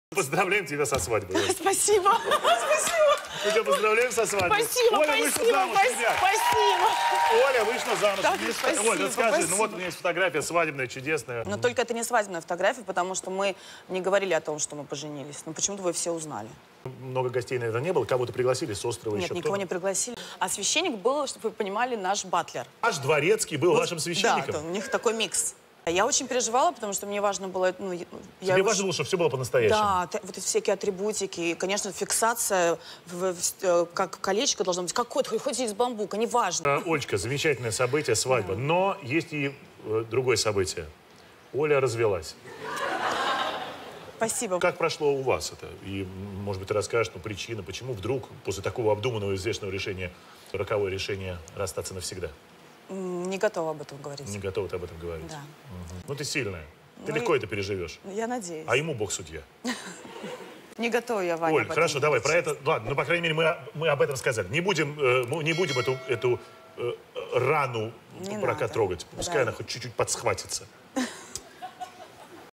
Ольга Бузова о Свадьбе и Разводе
Оля Бузова рассказала своим зрителям и поклонникам о том как и где проходила их свадьба, о которой, как они думали, никто не знал. Смогла ли она рассказать почему решили развестись навсегда?

Спасибо!